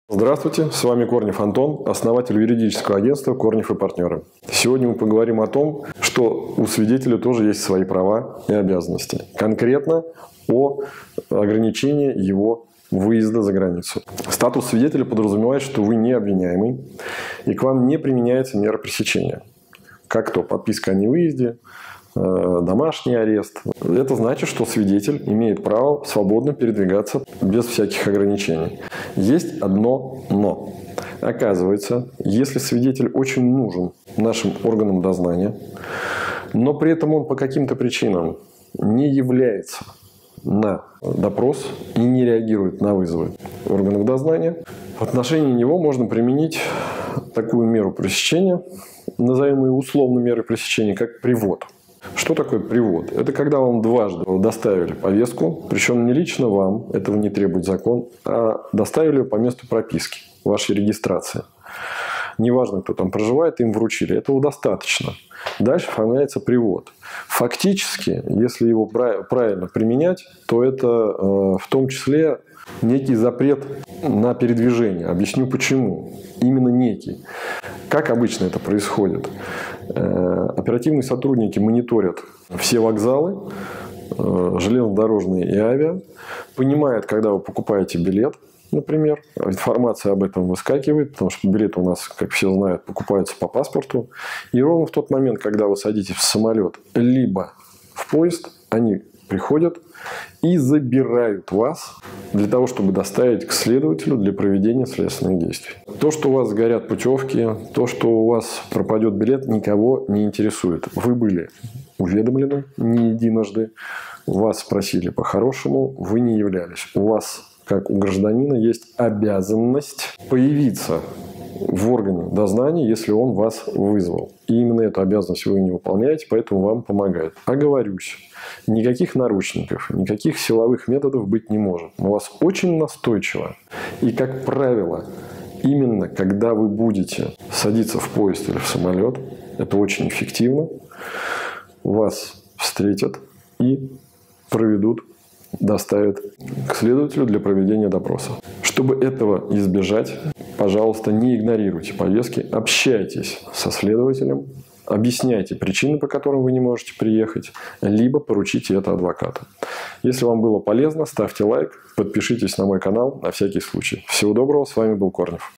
Права и обязанности свидетеля | Как происходит привод свидетеля на допрос
Добрый день! С вами Корнев Антон

Что будет, если свидетель на допросе не появляется, хотя нужные запросы до него отправлялись? Как состоится допрос свидетеля, если он попытается улететь или уехать? Будет ли запрет на выезд?

Если вы свидетель уголовного дела, обязательно знайте про права и обязанности свидетеля. Предупреждение об ответственности обязательно вам поступит, но нужно ли вам это? Зачем ждать привод свидетеля, когда вы собрались улететь за границу? Достаточно исправно исполнять обязанности свидетеля и являться вовремя на допрос к следователю.

Для коммерческих запросов писать на почту